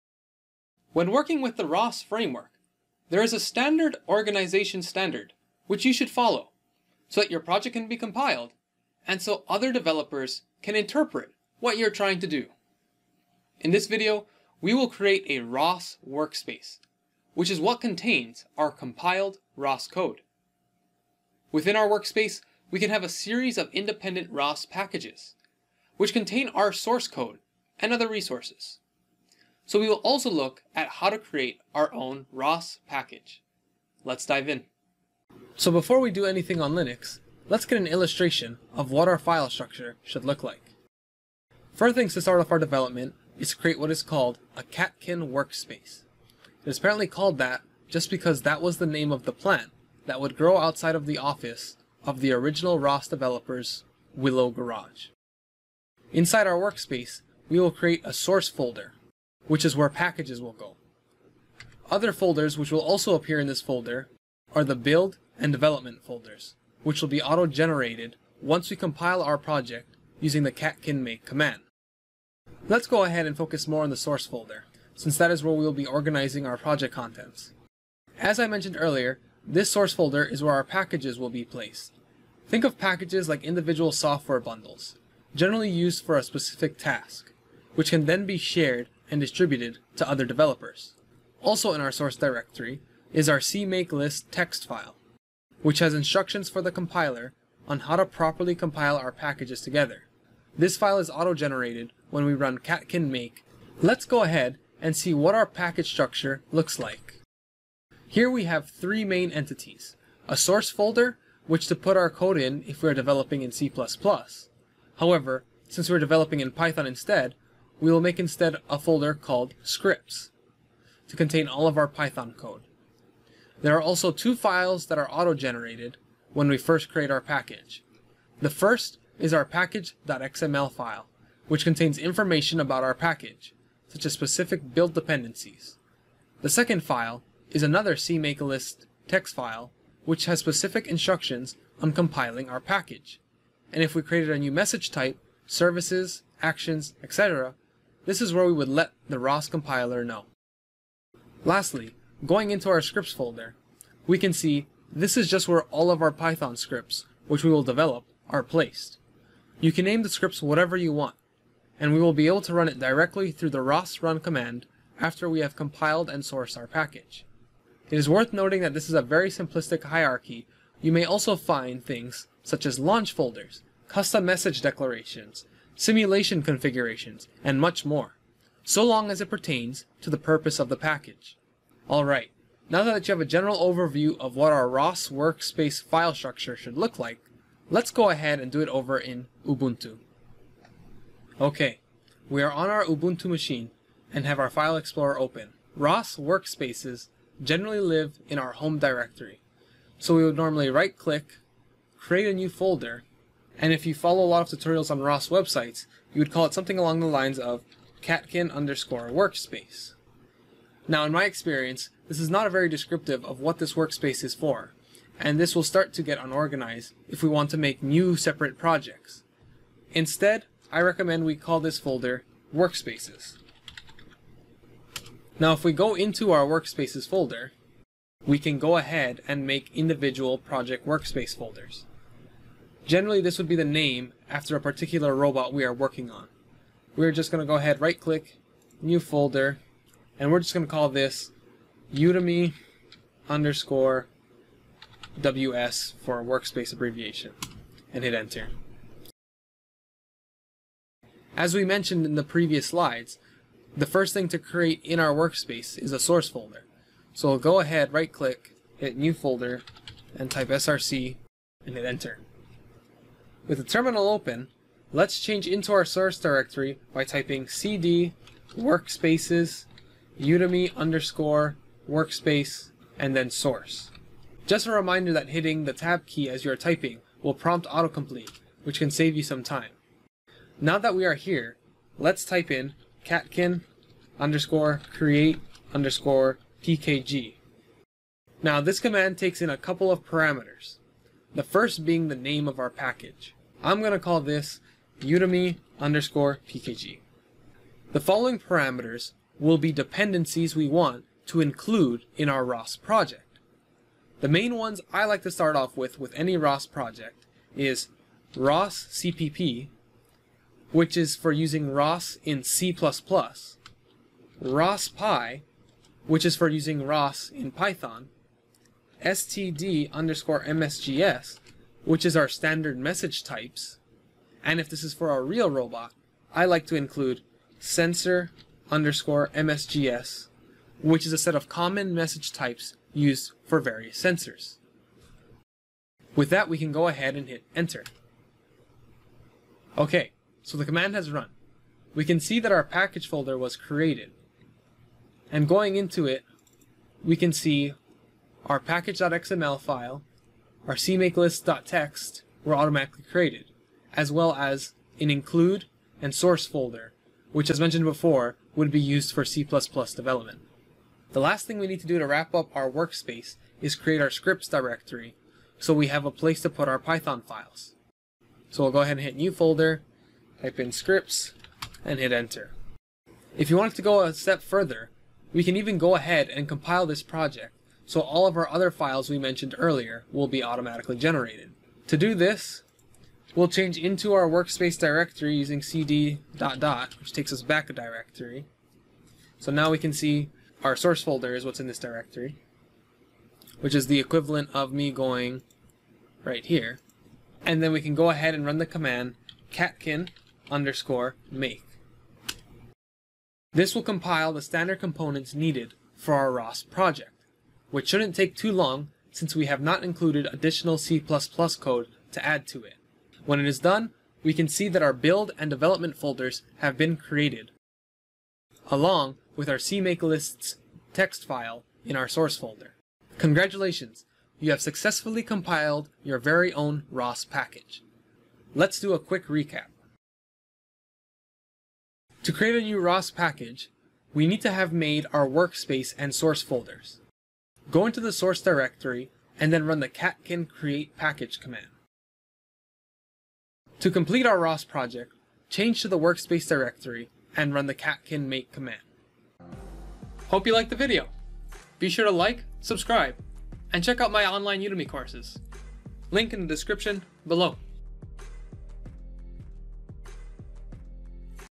Creating ROS Workspaces (and Packages) [ROS1 Noetic]
Let's go over how to properly organize your ROS workspaces, and create your very own ROS package.

You want to make sure you have your ROS code properly organized so other developers can understand what your code is for, and so ROS processes it properly when running catkin_make.